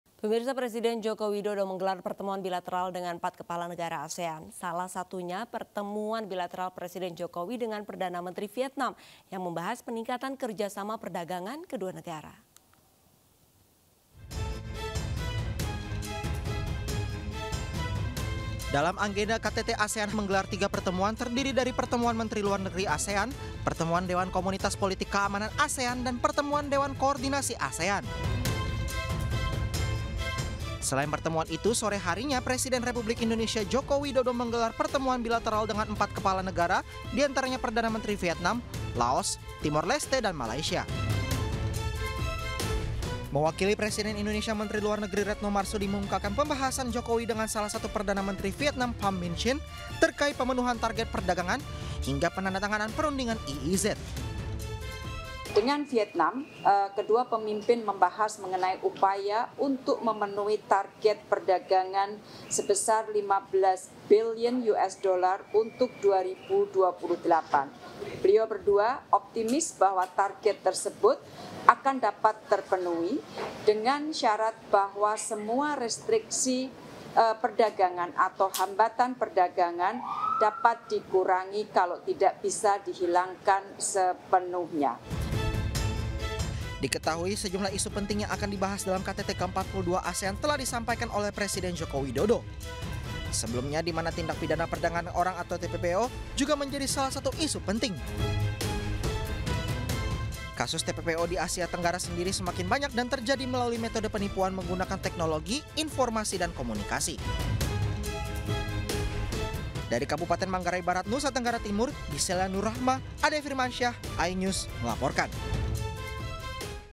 Presiden Jokowi Gelar Pertemuan Bilateral dengan Empat Kepala Negara
Presiden Joko Widodo (Jokowi) dijadwalkan melakukan pertemuan bilateral dengan empat kepala negara anggota ASEAN di sela-sela Konferensi Tingkat Tinggi (KTT) ke-42 ASEAN di Labuan Bajo pada 9-11 Mei 2023.. Negara yang dimaksud antara lain Vietnam, Laos, Timor Leste, dan Malaysia.

Berdasarkan informasi yang diterima oleh Infopublik.id menyebutkan, empat negara tersebut sudah dijadwalkan melakukan pertemuan bilateral dengan Presiden Jokowi saat rangkaian pertemuan ASEAN Summit ke-42 yang digelar di Labuan Bajo, Manggarai Barat, Nusa Tenggara Timur (NTT) pada Selasa (9/5/2023).